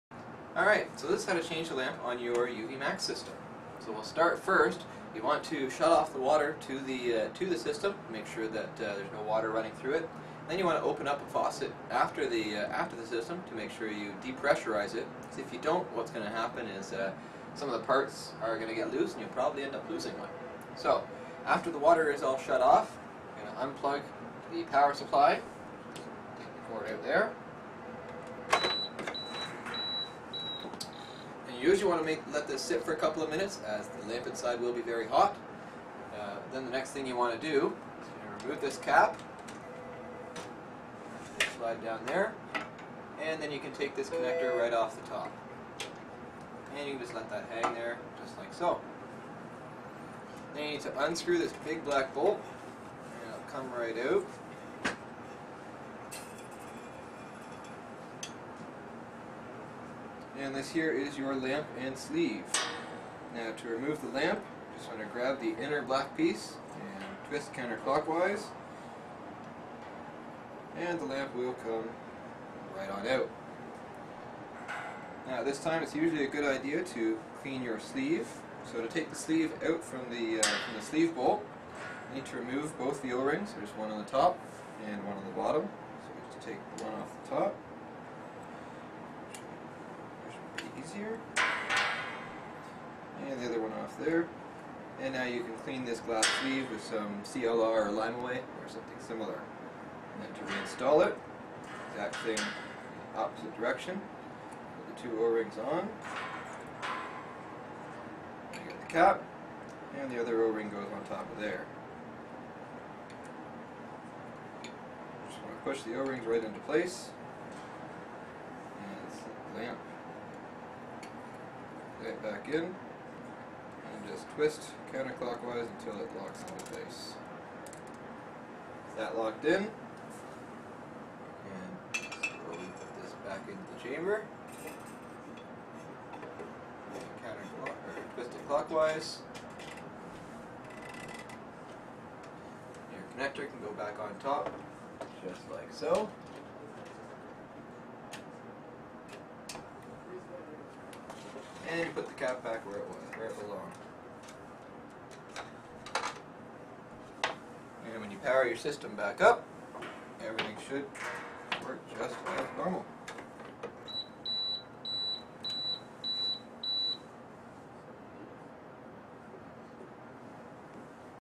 How to Change a Trojan / Viqua UltraViolet UV bulb or lamp.
- Easy step by step instructions on how to change a Trojan / Viqua UVMax Ultra Violet UV bulb, lamp, or sleeve.